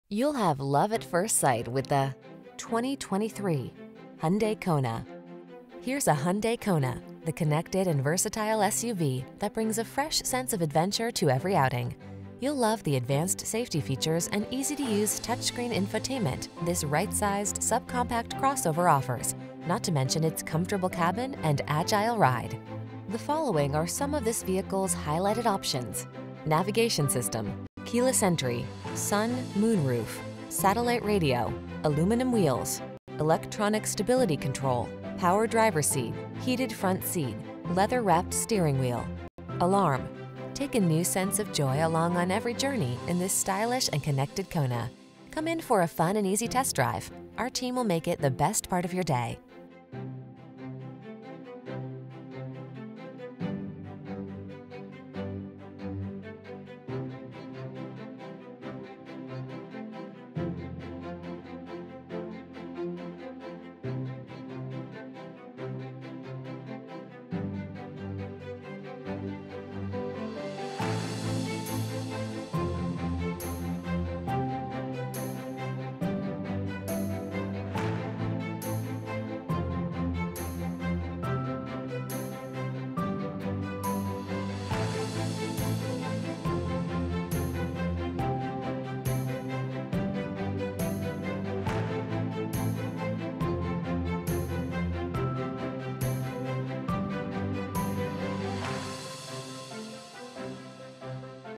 2023 Hyundai Kona North Augusta Aiken Augusta Grovetown Evans U965124R
Lunar White New 2023 Hyundai Kona SEL available in 5585 Jefferson Davis HWY, North Augusta, South Carolina at Terry Lambert Hyundai. Servicing the North Augusta, Aiken, Augusta, Grovetown, Evans SC area.

Kona SEL
2023 Hyundai Kona SEL - Stock#: U965124R

Lunar White 2023 Hyundai Kona SEL FWD IVT 2.0L 4-CylinderbrbrRecent Arrival! 30/35 City/Highway MPG